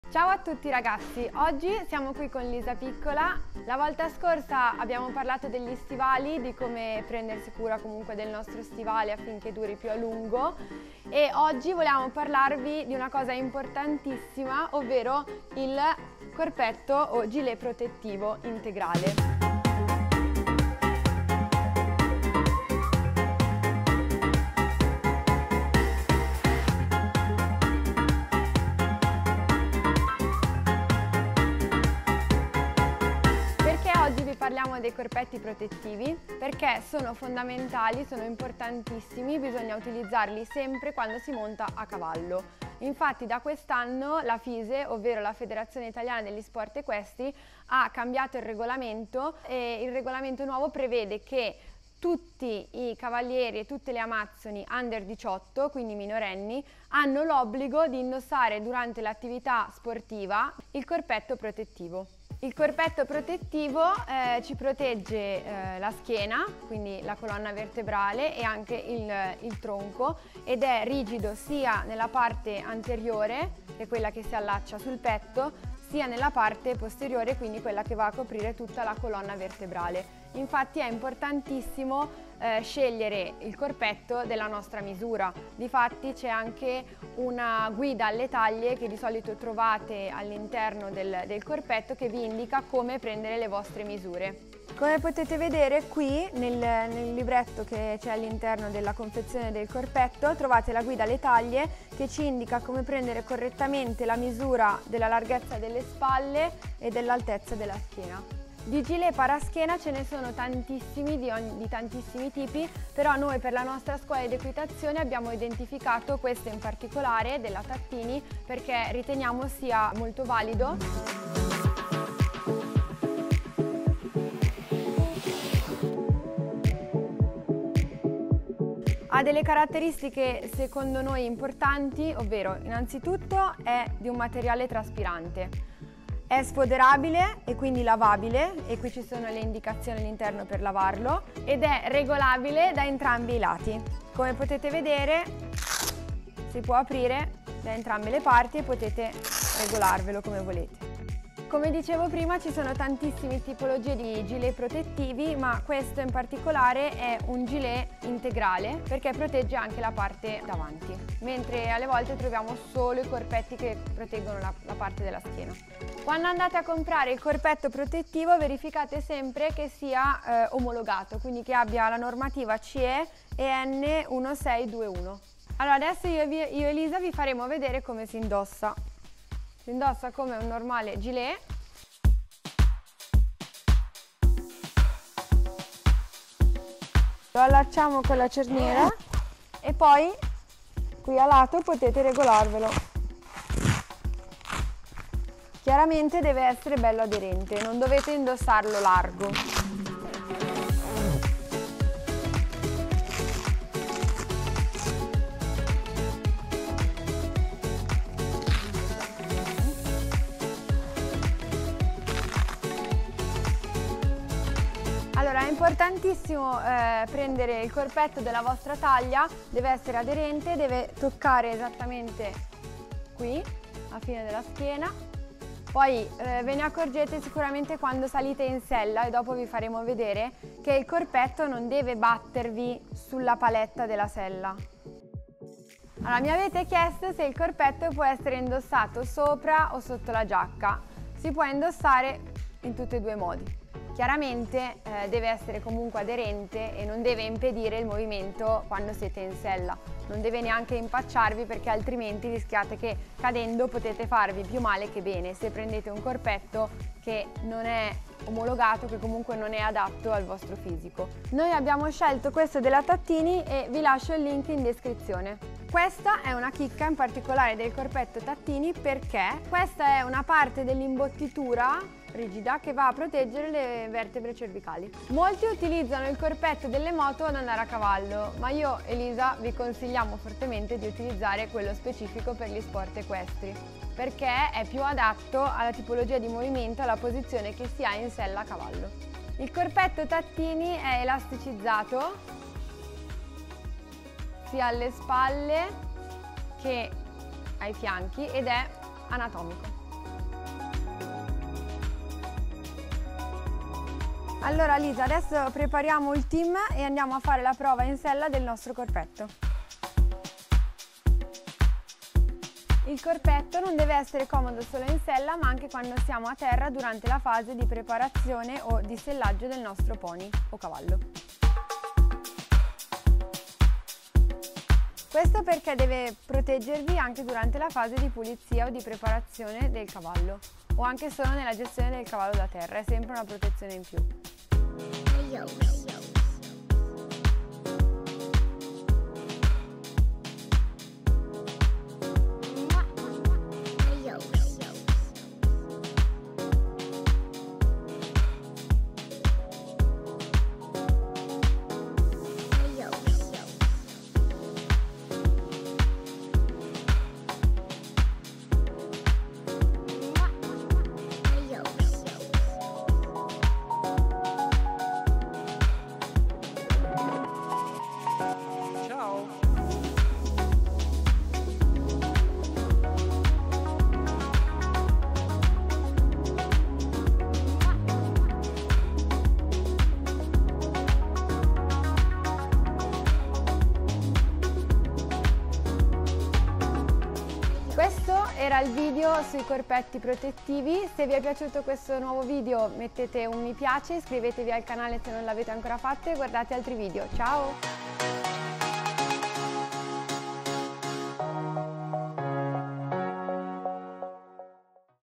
IL CORPETTO CHE PROTEGGE IN CASO DI CADUTE DAL CAVALLO
In questo video parleremo di SICUREZZA a cavallo.
In particolare sui corpetti protettivi, vedremo quali e come sceglierli.
Io per la mia scuola ho scelto le protezioni Tattini.
Buona visione!